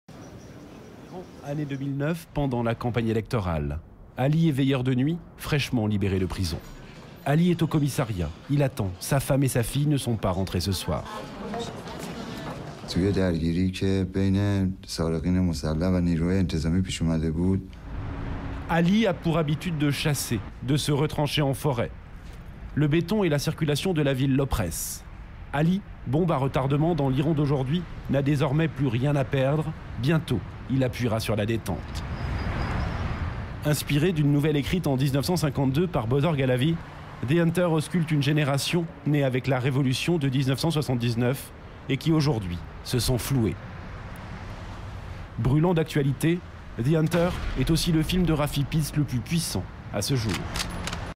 THE HUNTER - RAFI PITTS
Journal de ARTE (en français), "The Hunter" un film de "Rafi Pitts",